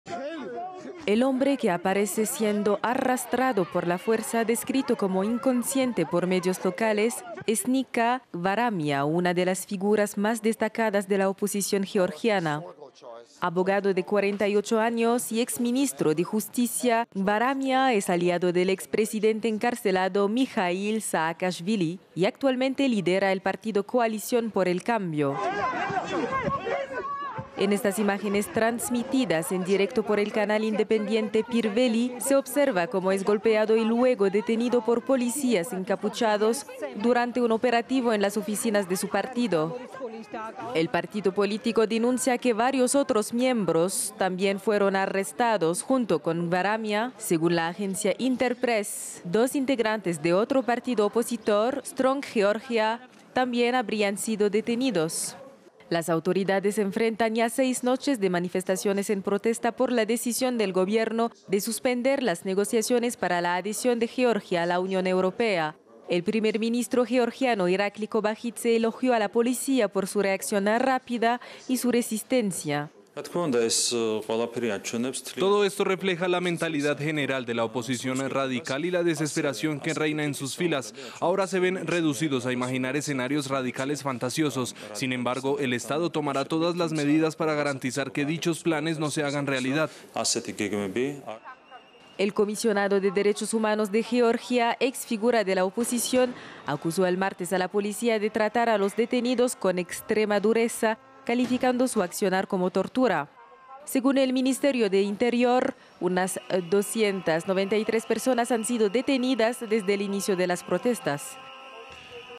Policía de Georgia allanó sedes de partidos opositores y detuvo a varios de sus miembros
Georgia vivió su séptimo día de protestas pro Unión Europea. Durante la más reciente jornada, la Policía allanó la sede de un partido opositor y detuvo a su líder; según informes, otros opositores habrían sido arrestados junto a él. Los manifestantes siguen en las calles rechazando la decisión del Gobierno de pausar las negociaciones para la adhesión de Georgia al bloque europeo.